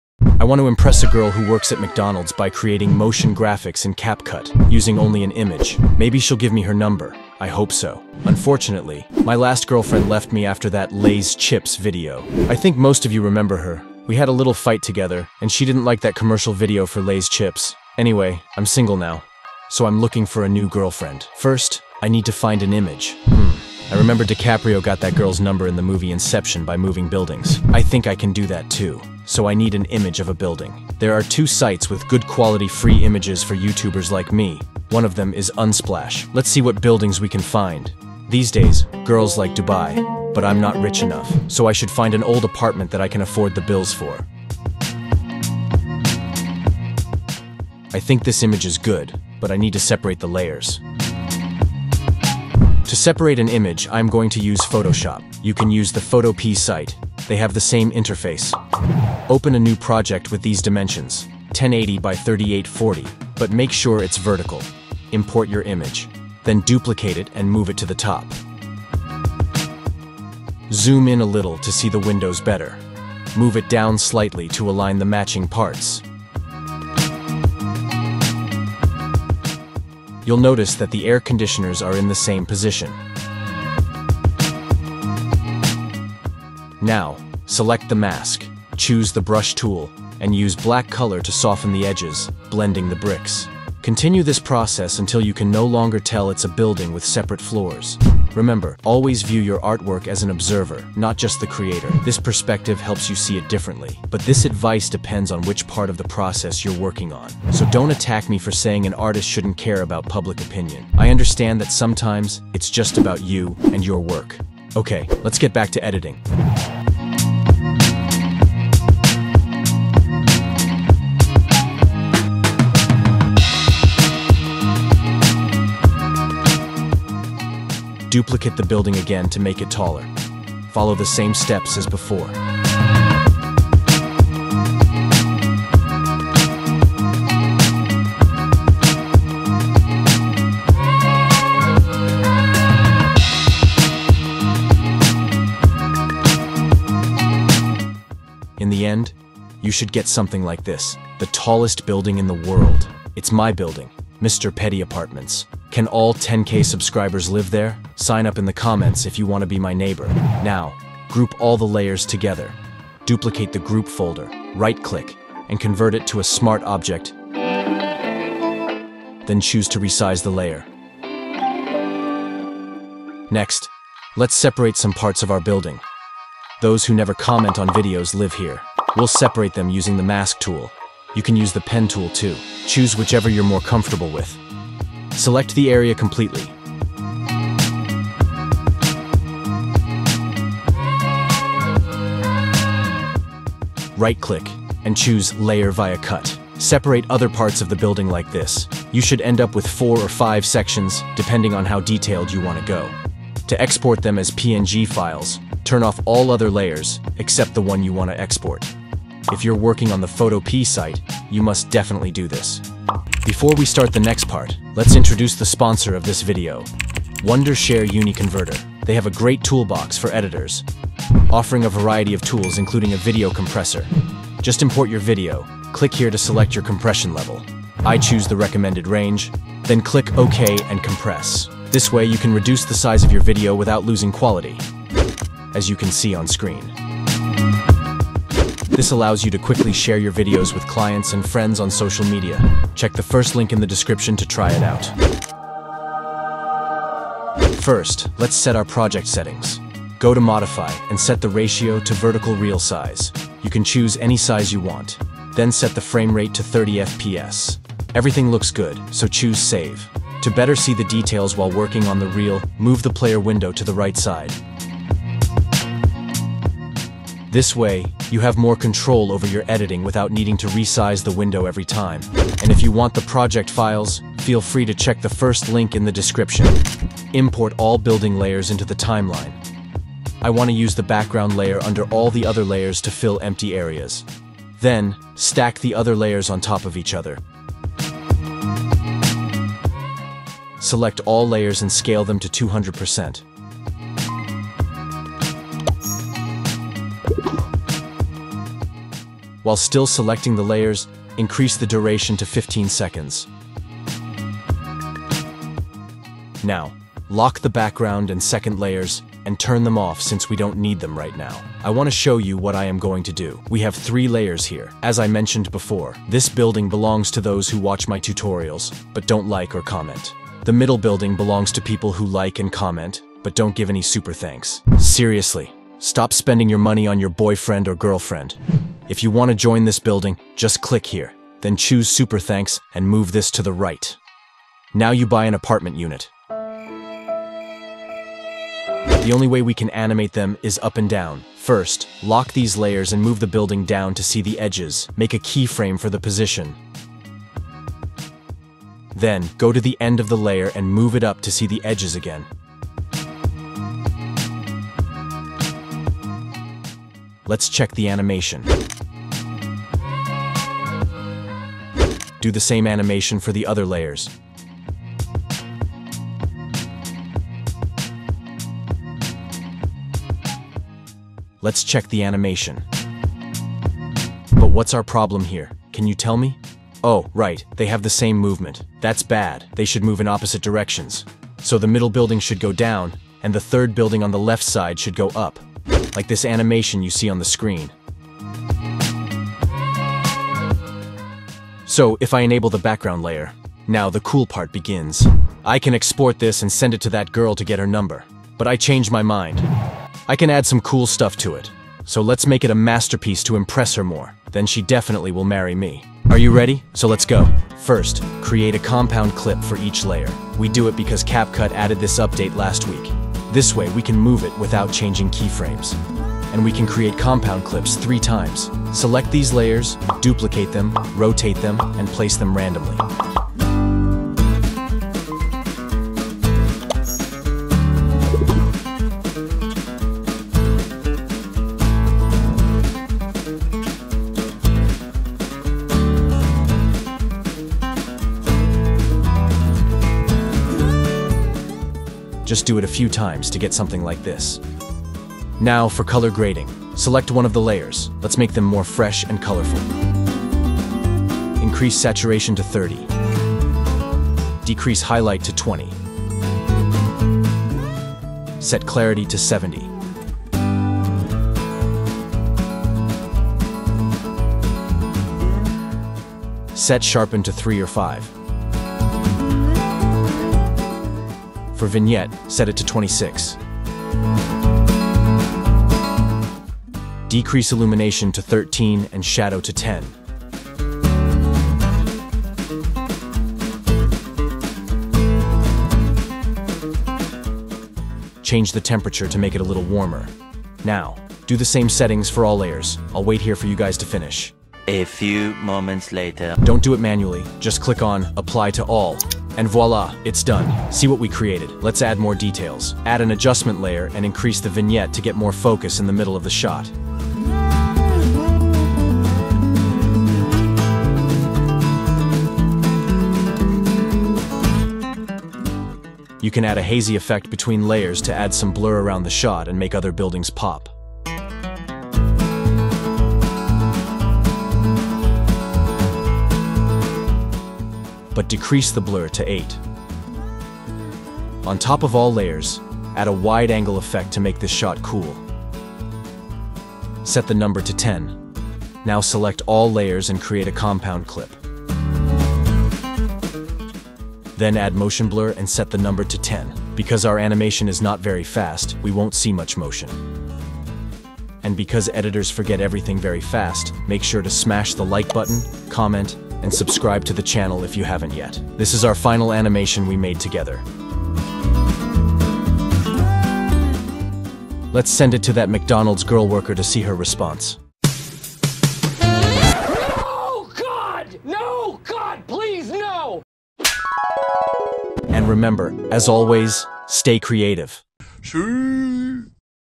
How I Make Creative Motion Graphics In CapCut (Full Tutorial)
In this video, you'll learn how to create stunning "Motion Graphics" using CapCut to impress Emma Stone. This step-by-step tutorial is perfect for beginners looking to enhance their editing skills!

as Always...
+ STAY Creative!

Chapters :
00:00 Step 1: McDonald's Girl Worker
00:09 Step 2: Why She Left Me?
00:22 Step 3: How to Find Copyright-Free Images on Google (Step 1)
00:53 Step 4: Separating Buildings
03:34 Step 5: Sponsor: Wondershare UniConverter
04:05 Step 6: How to Make Motion Graphics on CapCut
07:26 Step 7: How to Color Grade in CapCut
09:11 Step 8: I Got Her Number